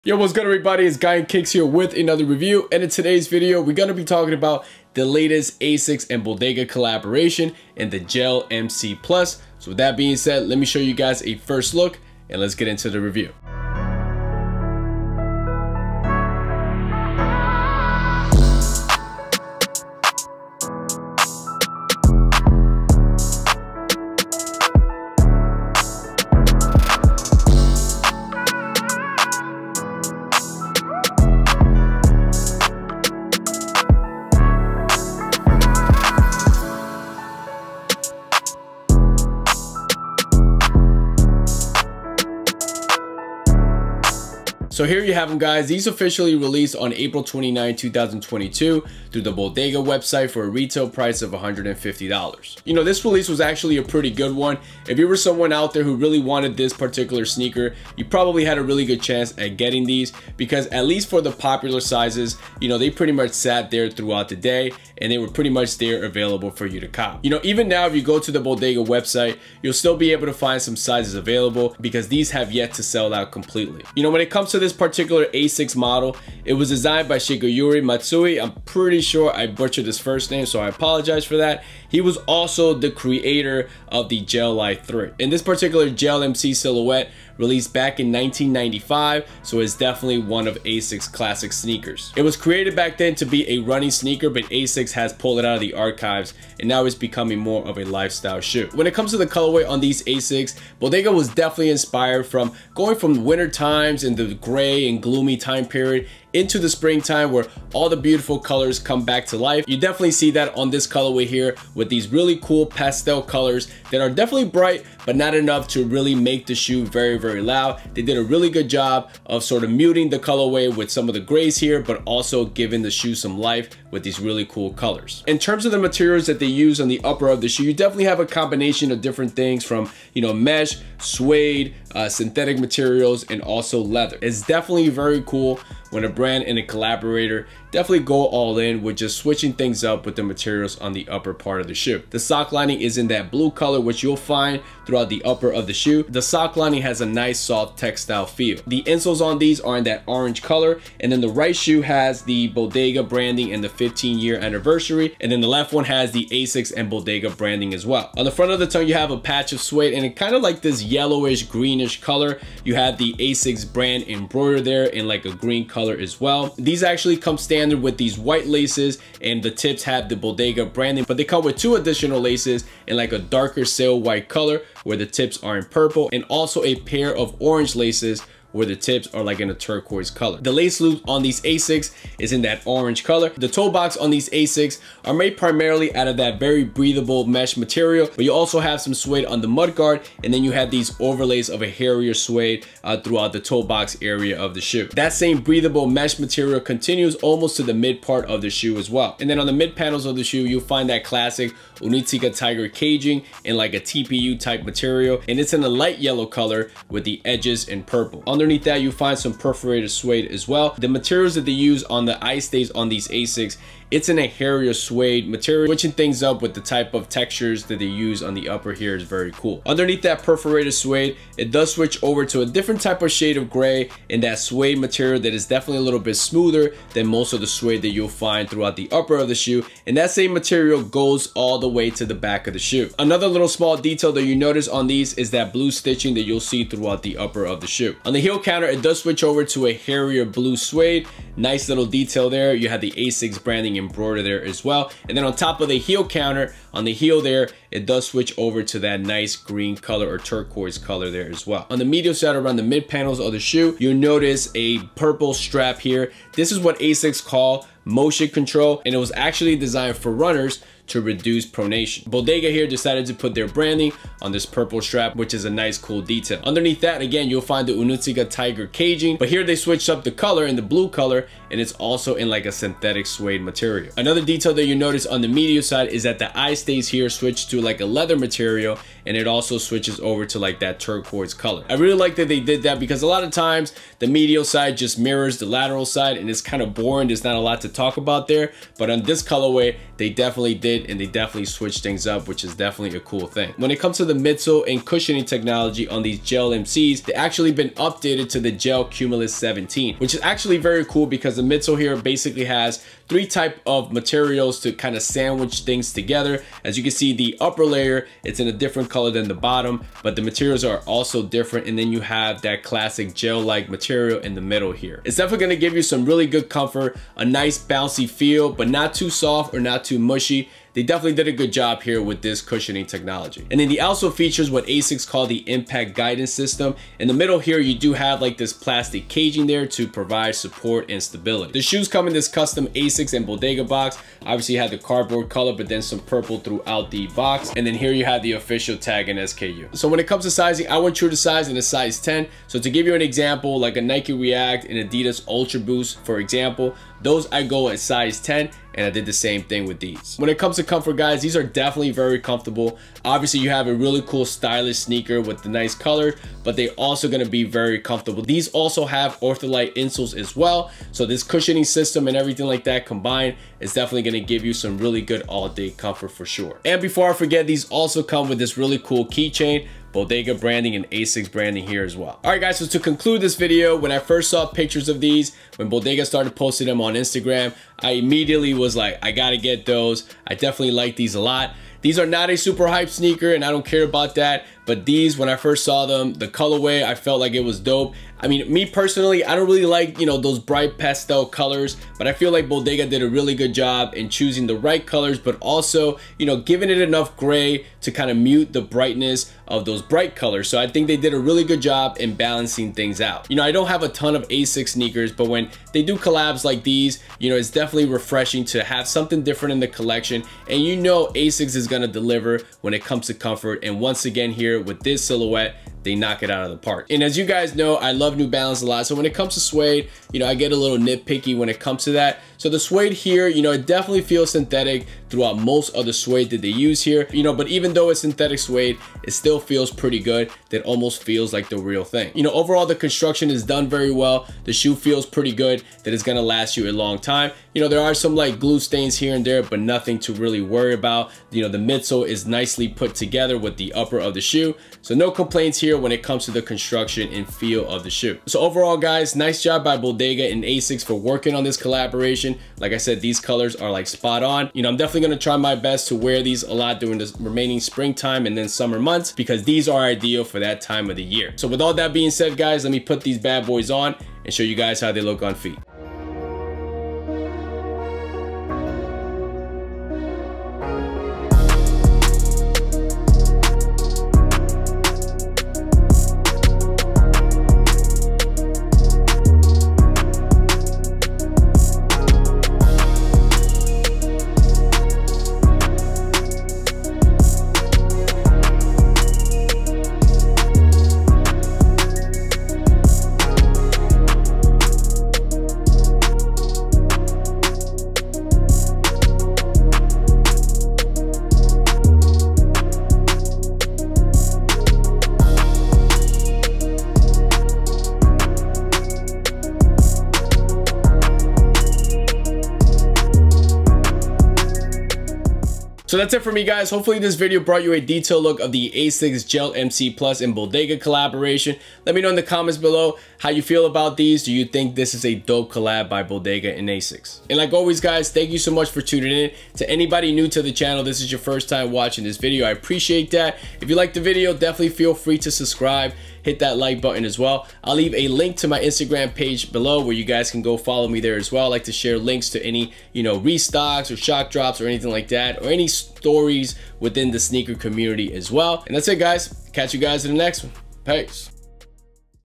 Asics Gel-MC Plus x Bodega Review - Perfect spring/summer shoe?
Hey guys, in this review I go over the latest Asics x Bodega collaboration. This silhouette was released back in 1995 and Bodega chose a great comfy shoe, and added some really nice colors for spring/summer months.